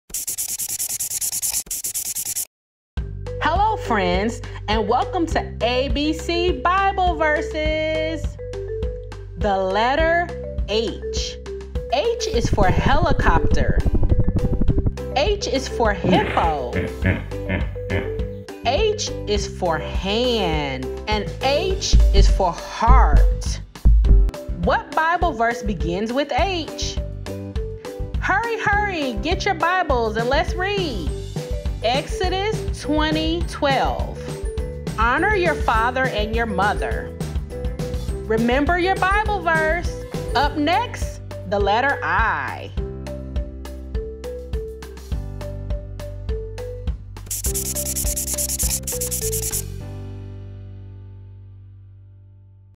Xtreme Kidz Preschool | ABC Bible Train Letter H
Xtreme Kidz Studios presents to you the ABC Bible Train for our younger Xtremers (Pre-K).  Each week our Train will help teach a letter with a word association and a Bible Verse to remember with that Letter!

We want to help Educate, Entertain, and Evangelize to our youngest members!  Join us as we learn our letters and bible verses together!